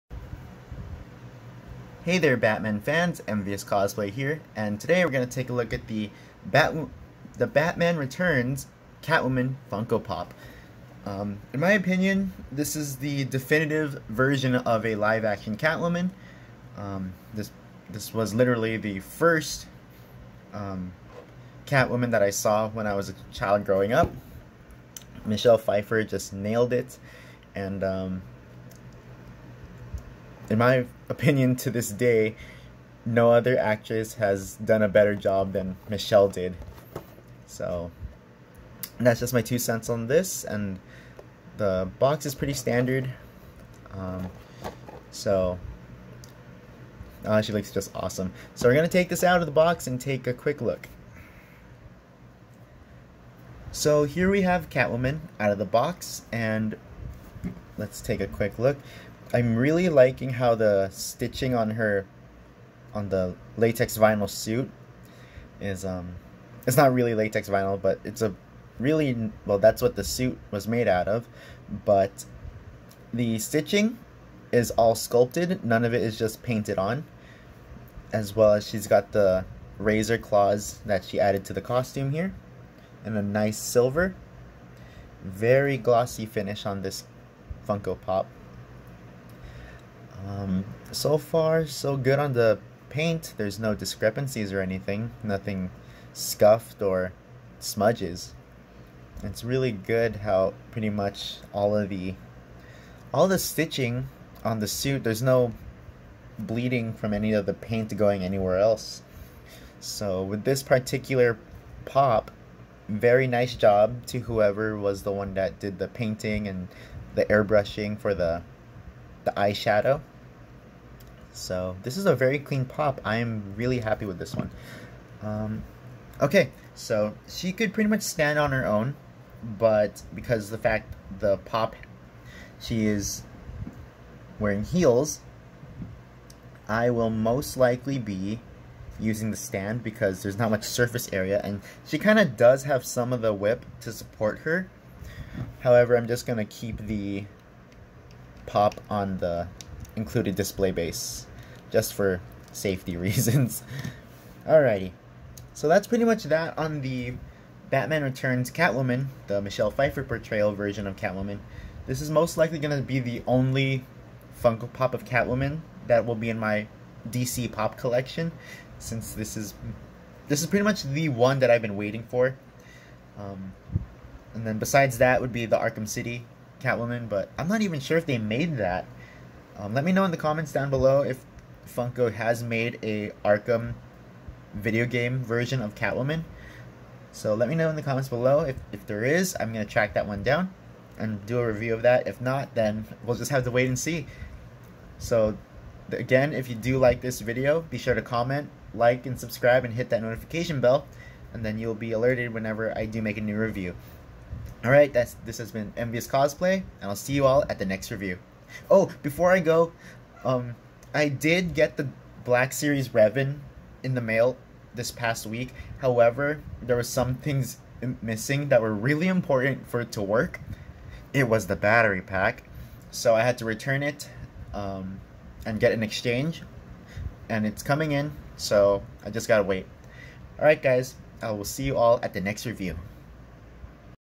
Catwoman (Batman Returns) Funko Pop REVIEW
"I'm Catwoman. Hear me roar."

I was at a Hot Topic yesterday with a friend of mine and I finally found a Pop of my all time favorite version of Catwoman in a store's Funko section! Which is your favorite version of Catwoman?

Social media links:
☆ MVious Cosplay